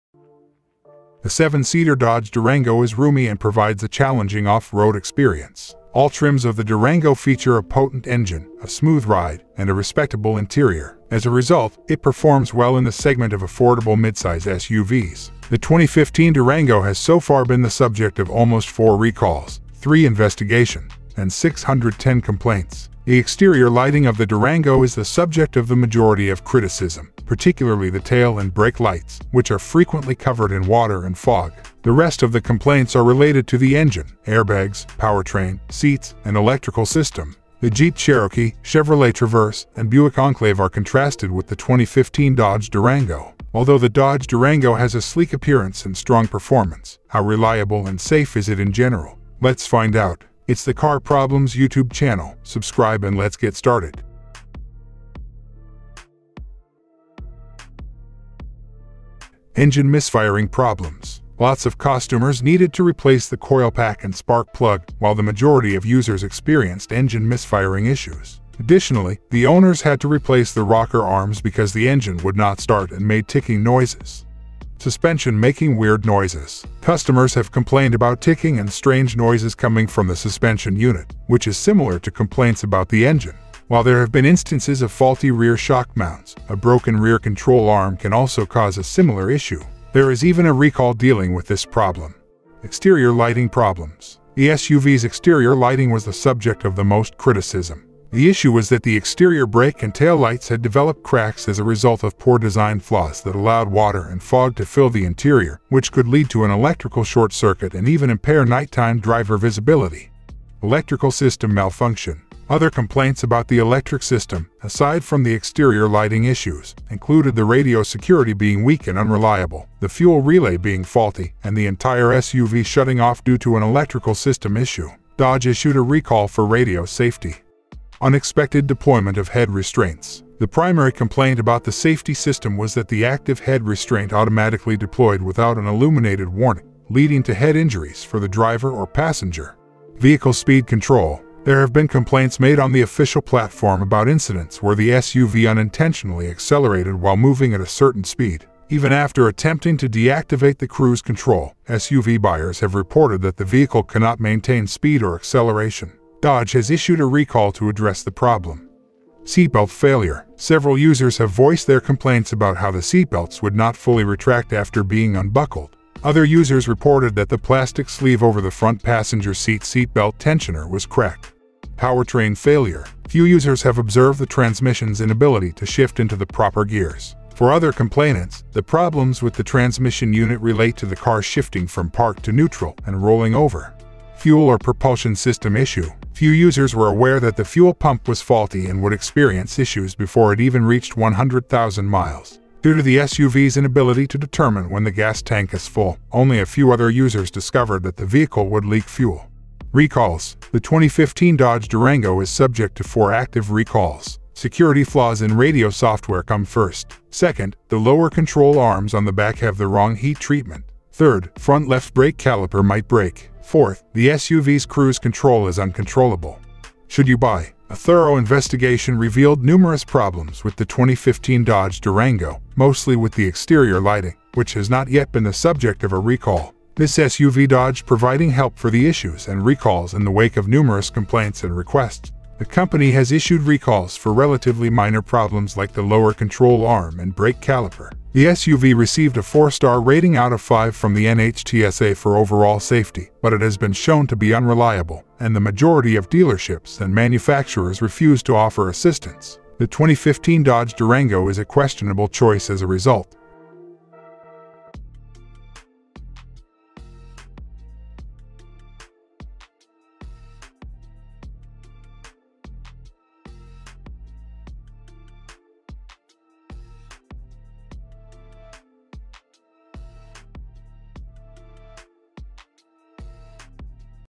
2015 Dodge Durango Problems, Recalls, Reliability. Should you buy it?
Although the Dodge Durango has a sleek appearance and strong performance, how reliable and safe is it in general? Let's find out!

Timecode:
00:00:00 - Blah-blah...
00:00:59 - Engine misfiring Problems
00:01:14 - Suspension making weird noises
00:01:32 - Exterior Lighting Problems
00:01:50 - Electrical system malfunction
00:02:07 - Deployment of head restraints
00:02:19 - Vehicle speed control
00:02:38 - Seatbelt Failure
00:02:51 - Powertrain failure
00:03:03 - Fuel / Propulsion system issue
00:03:19 - Recalls
00:03:36 - Should you buy?